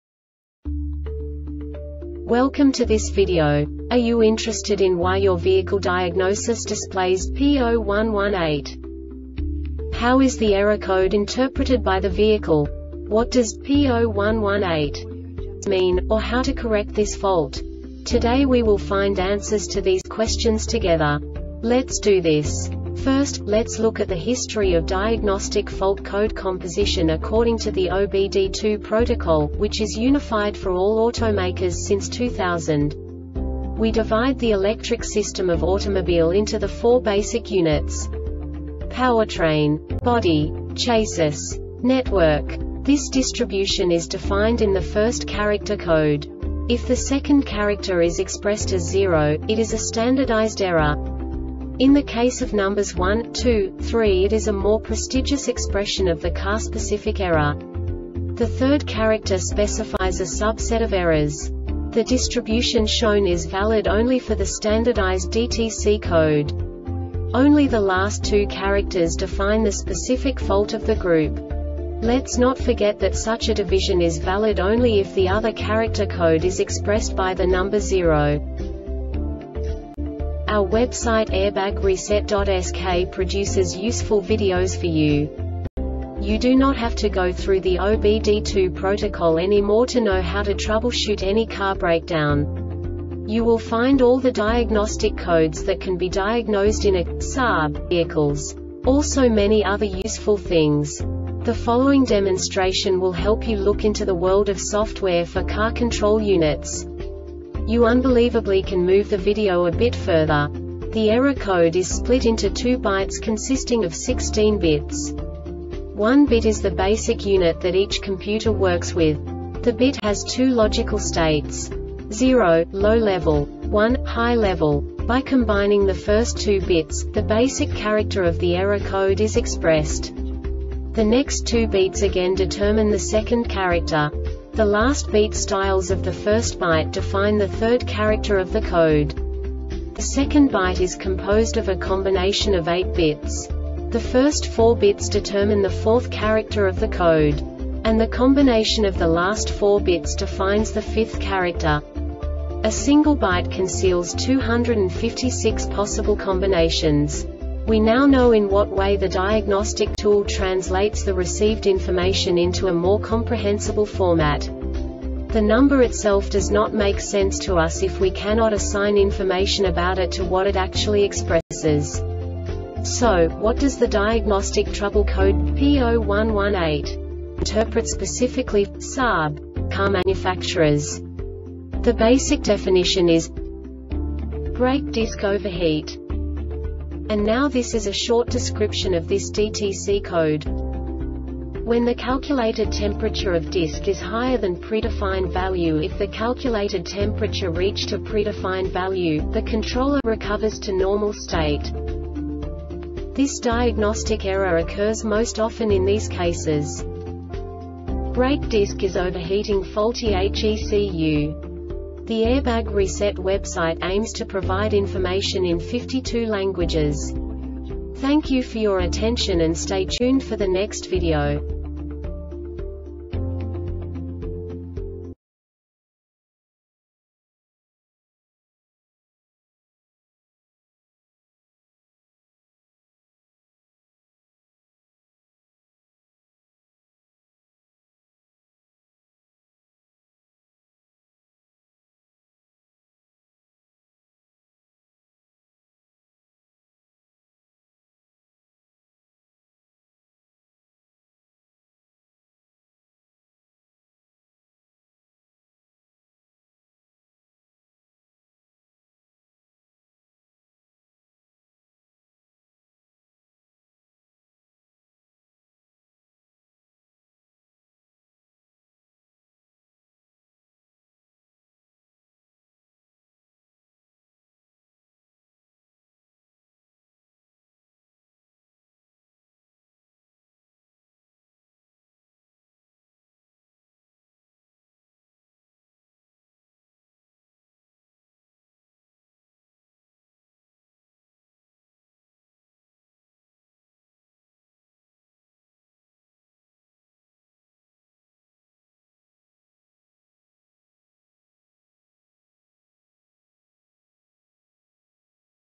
DTC Saab P0118 Short Explanation
Contents:
0:21 Basic DTC analysis according to OBD2 protocol standard.
1:48 Insight into programming
2:58 A diagnostically understandable description of the Diagnostic Trouble Code
3:05 Basic error definition

Make:
Saab
Code:
P0118
Definition:
Brake Disc Overheat
Description:
When the calculated temperature of disc is higher than predefined value.If the calculated temperature reach to predefined value,the controller recovers to normal state.
Cause:
 Brake disc is overheatingFaulty HECU